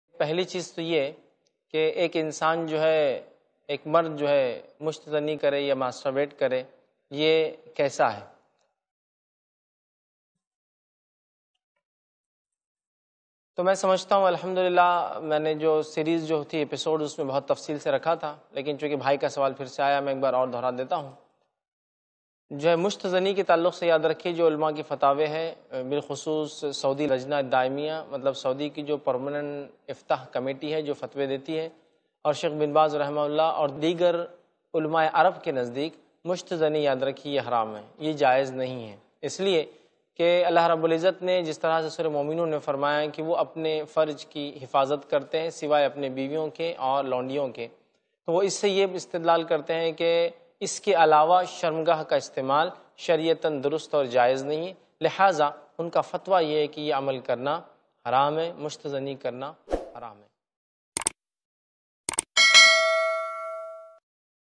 Masturbation karna Islam mei haram ya halal hai ? || By Afv. faiz sayyed
Shorts Clip Bayan :
BY__ADVOCATE FAIZ SAYYED
۰۰۰۰۰۰۰۰۰۰۰۰۰۰۰۰۰۰۰۰۰۰۰۰۰۰۰۰۰۰۰۰۰۰۰۰۰۰۰۰۰۰۰۰۰۰۰۰۰۰۰۰۰۰۰۰۰۰۰۰۰۰۰۰۰۰۰۰۰۰۰۰۰۰۰۰۰۰۰۰۰۰۰۰۰۰۰۰۰۰۰۰۰۰۰۰۰۰۰۰۰۰۰۰۰۰۰۰۰۰۰۰۰۰۰۰۰۰۰۰۰۰۰۰